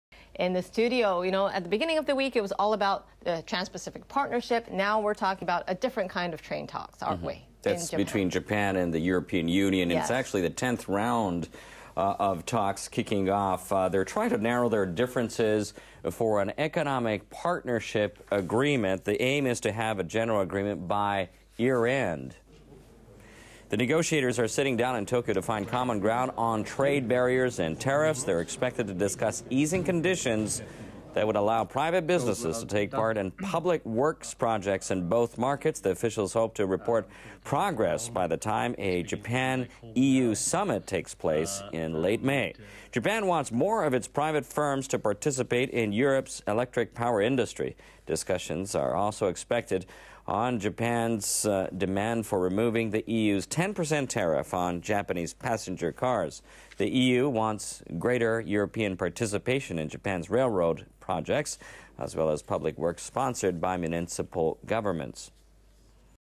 FSIFX Forex News Desk: Japan, EU begin EPA talks in Tokyo
Trade officials of Japan and the European Union have started their 10th round of free trade talks.